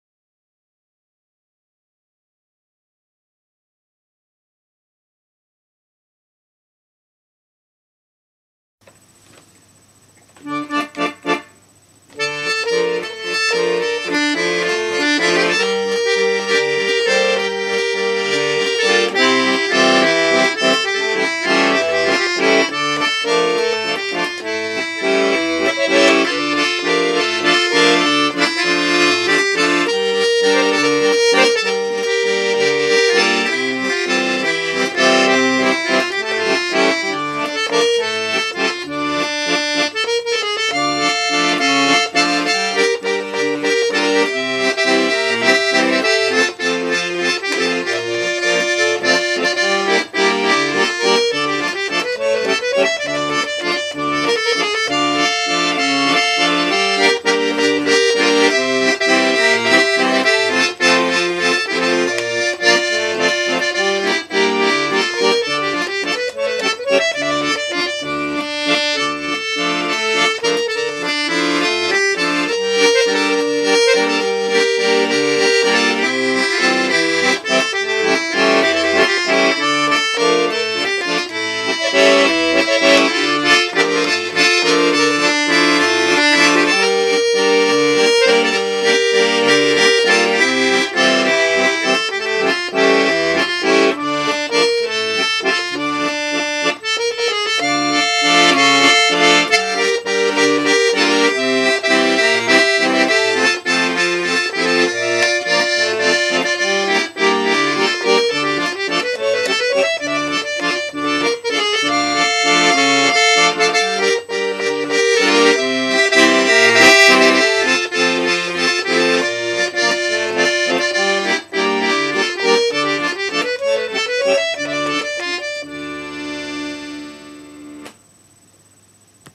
Cafe Waltz.mov
A French Cafe waltz in Dm from the playing of Marc Perrone - a personal hero of mine on the melodeon.